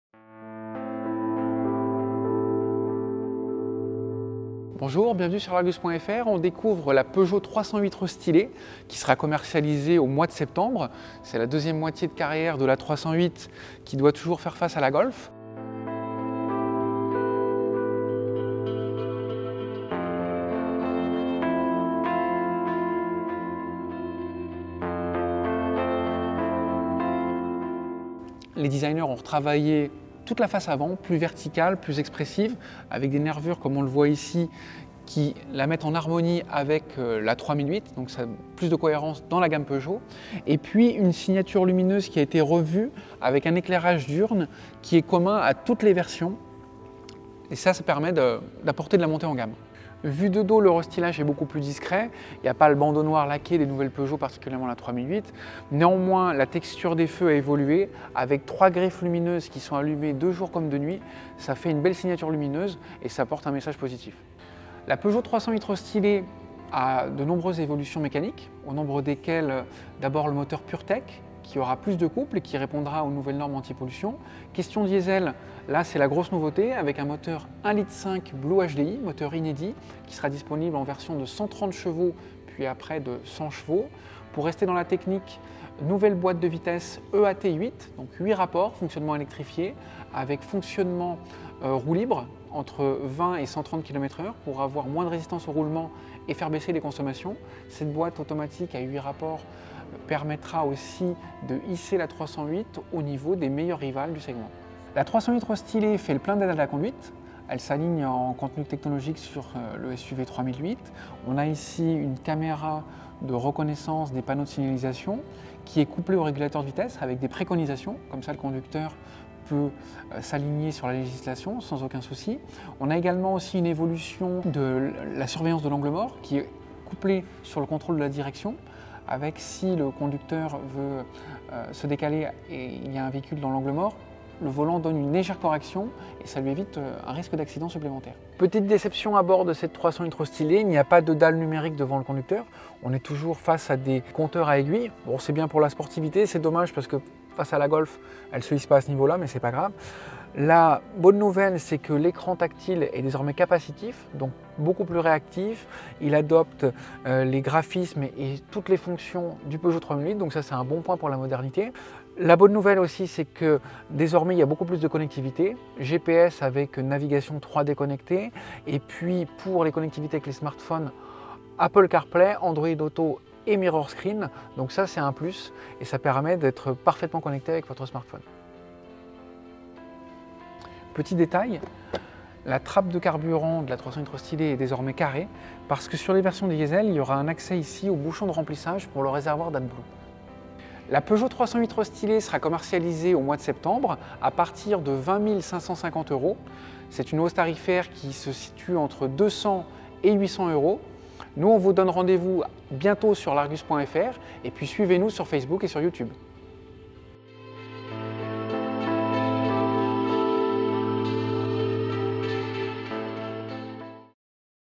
Nouvelle Peugeot 308 (2017) : la version restylée en détail.
Après quatre ans de carrière, la berline compacte n°1 des ventes en France passe par la case restylage. De légères évolutions esthétiques qui s’accompagnent d’un enrichissement de son bagage technologique et d’une refonte de la partie mécanique. L’argus.fr vous invite à découvrir en détails cette nouvelle Peugeot 308.